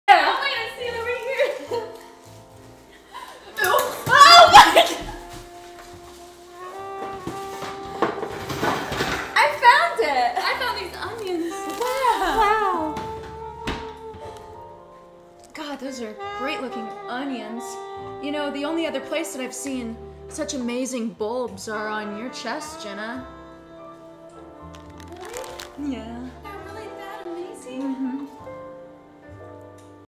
Meowri & PeachJars- Slapstick and Sexy Bulbs
They're OK! And they found some big, sexy, bulbous onions. They're sooo big and round...